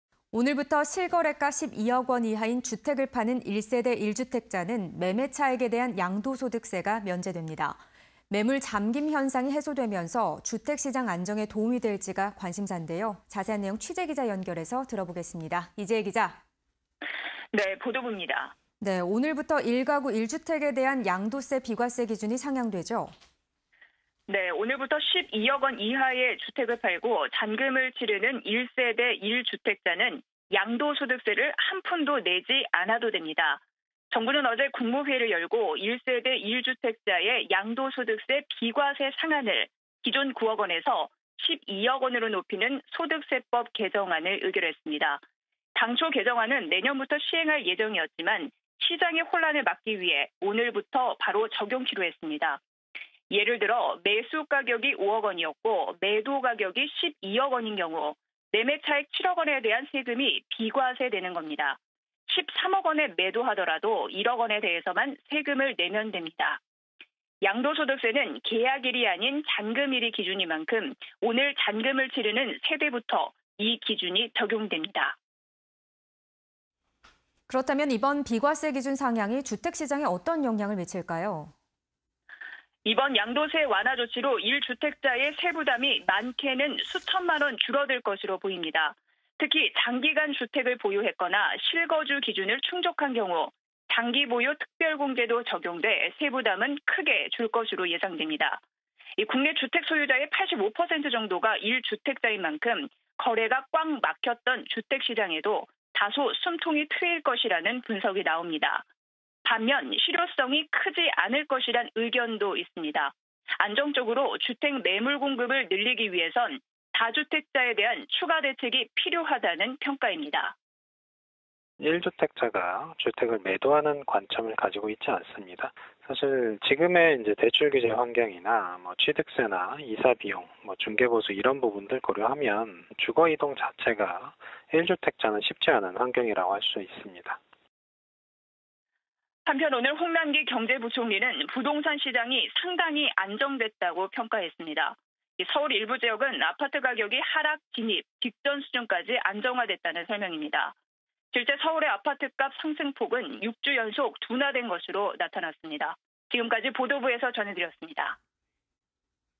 오늘부터 1주택 양도세 비과세 기준 상향… 주택시장 영향은? (20211208)
# 오늘부터 1주택 양도세 비과세 12억원... 수천만원 줄 수도
- 잔금 청산일·등기일 중 빠른 날 기준
- 주택시장 추가 매물 공급 기대... 다주택자 추가 대책 요구도
- 홍남기 “서울 일부 아파트값, 하락 직전 수준... 안정화 흐름 양상”